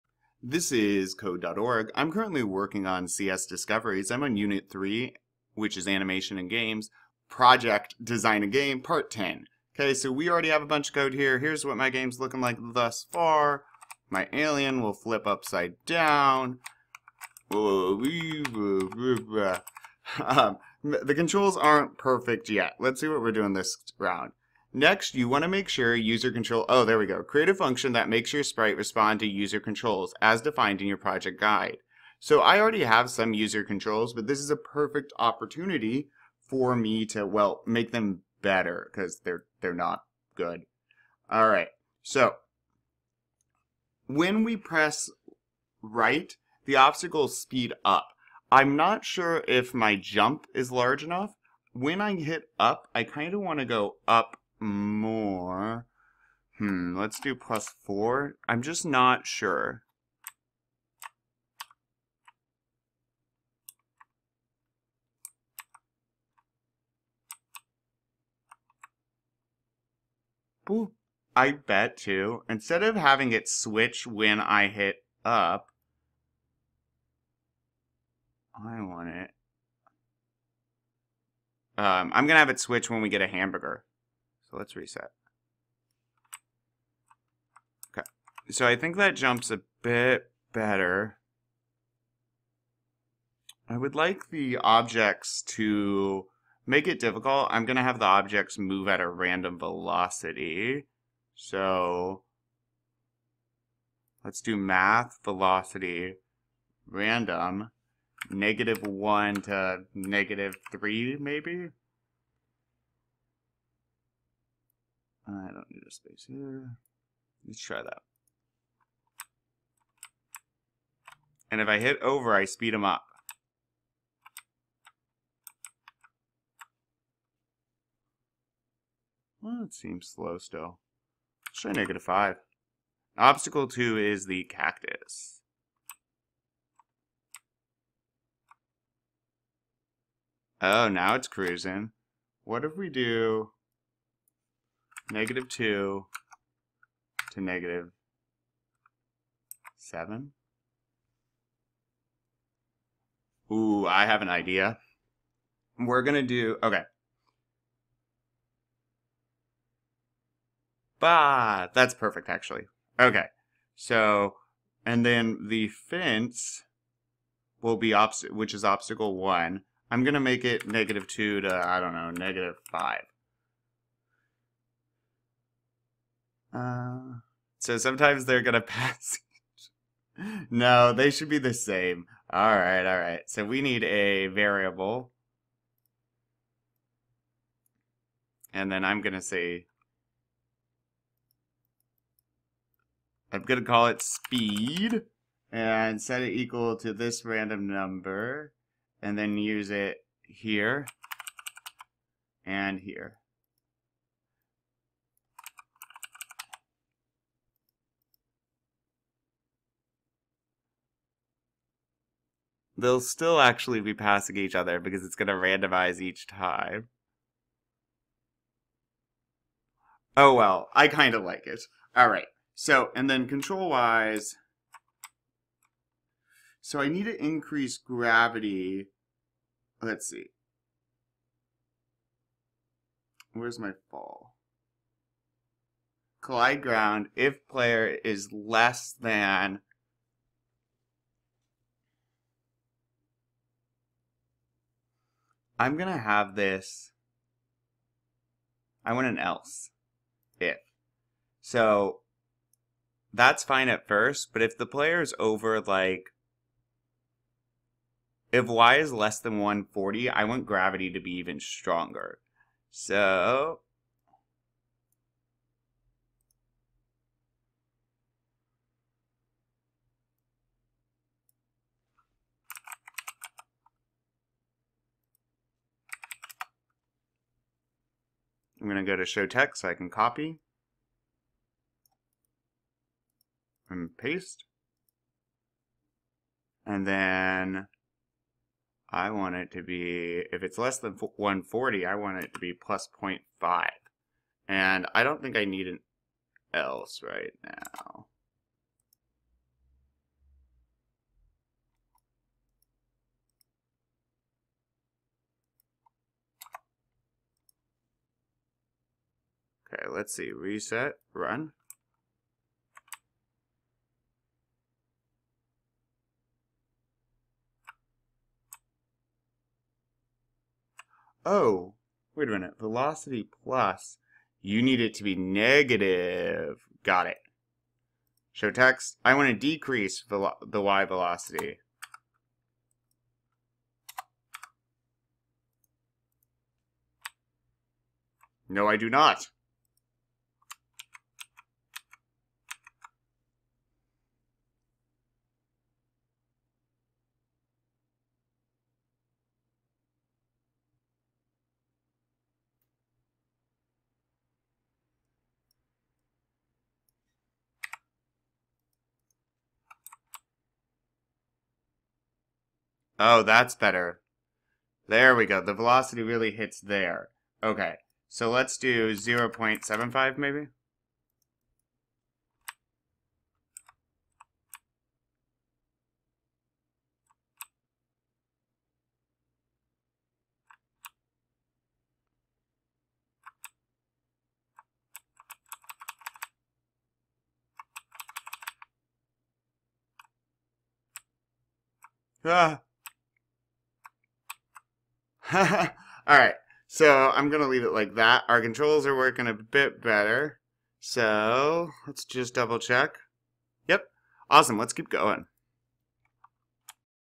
Code.org Lesson 28.9 Project - Design a Game | Tutorial with Answers | C.S. Discoveries Unit 3
Project  - Design a Game is a lesson from Interactive Animation and Games, unit 3 of Code.org's C.S. Discoveries course. This course is often used in middle and high school computer science classrooms. Each part of the course challenges students to accomplish a new task while discovering the principles of computer science. In this video, I walk through step-by-step how to get the right answer for this puzzle.